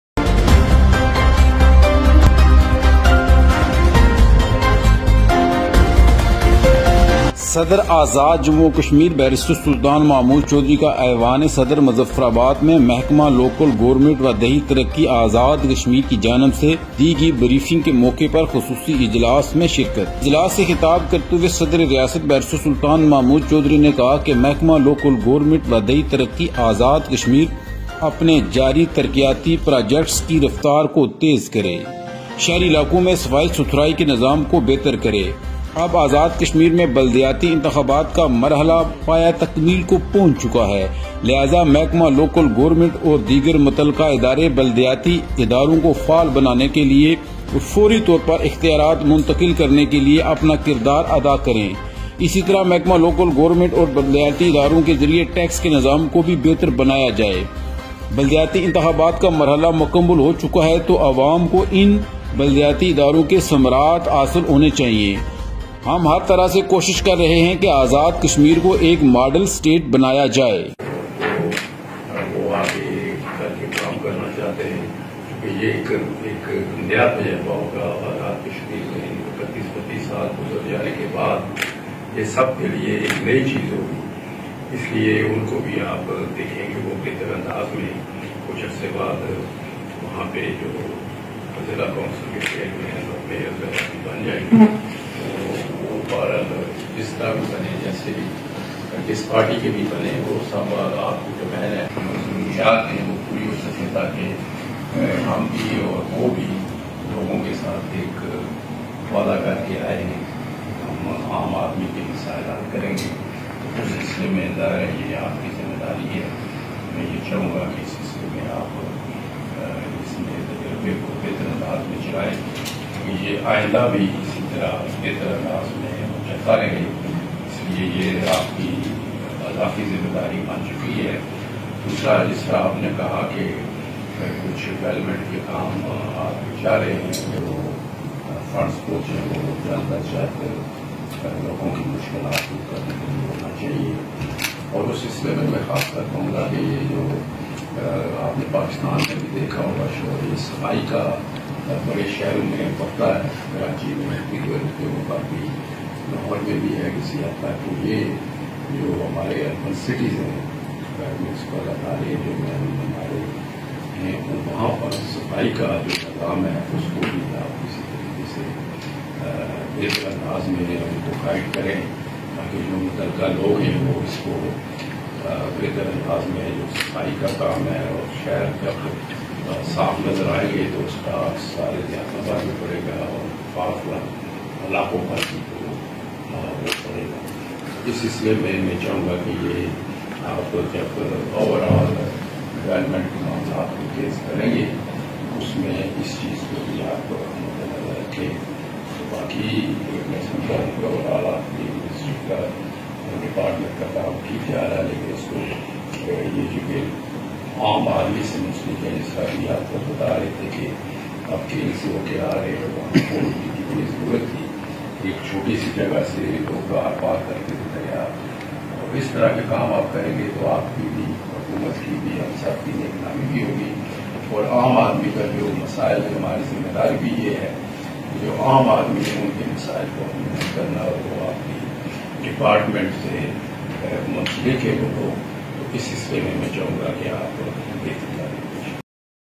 بیرسٹر سلطان کا محکمہ لوکل گورنمنٹ و دیہی ترقی آزادکشمیر کی جانب سے دی گئی بریفنگ کے موقع پر خطاب
صدر آزاد جموں وکشمیر بیرسٹر سلطان محمود چوہدری کا ایوان صدر مظفرآباد میں محکمہ لوکل گورنمنٹ و دیہی ترقی آزادکشمیر کی جانب سے دی گئی بریفنگ کے موقع پر خطاب بارے بیرسٹر سلطان میڈیا سیل انٹر نیشنل آفیشل کی خصوصی رپورٹ
Report Written by Naseer Ahmed
Voice
Naseer Ahmed
Video Editing Adil ch
Report released by Barrister Sultan media cell international official